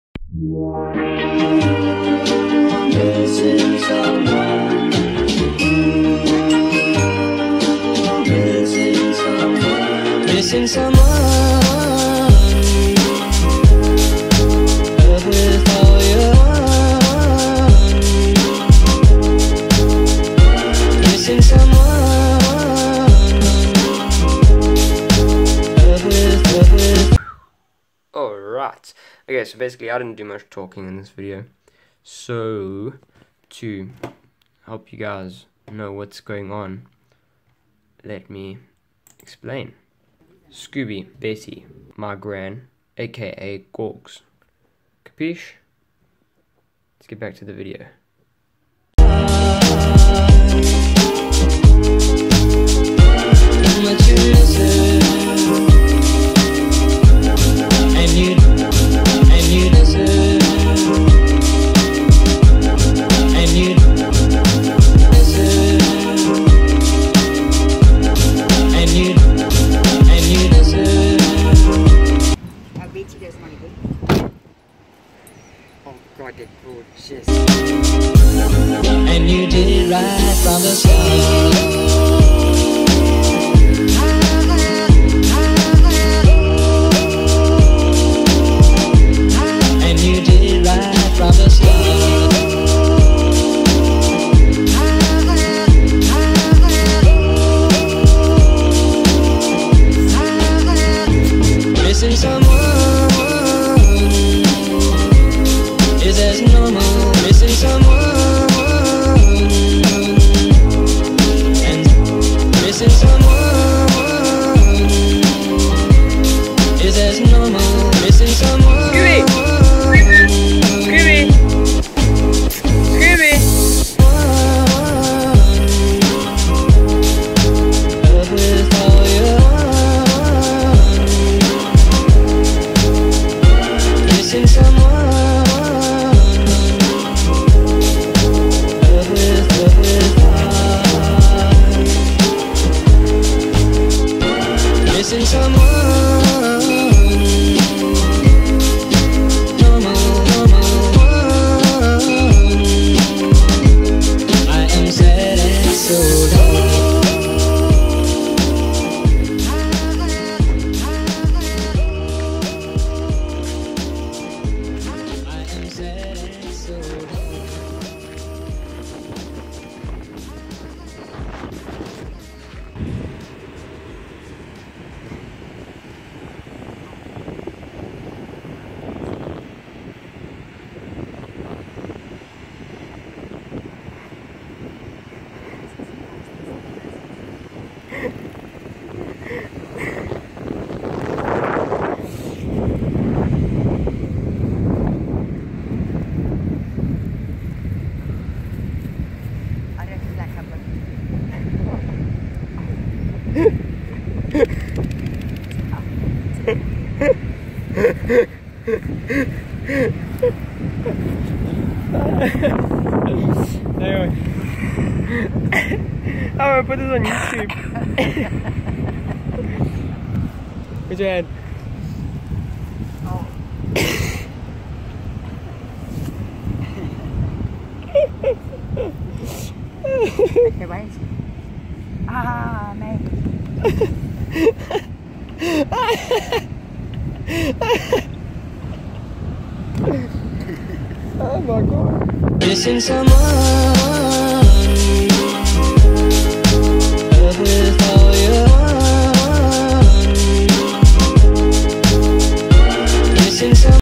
Old Lady Falls Down Hill EPIC FAIL __ VLOG 4
This video contains real footage of an old lady sliding, yes sliding, down a bank at the beach... She can't get up and it's absolutely hilarious.

So If you couldn't tell by the video, I went to the beach with my gran "Gogs", and her two dogs; Scooby and Betty.

my website... not that anyone cares--- dylanrhphotography.wordpress.com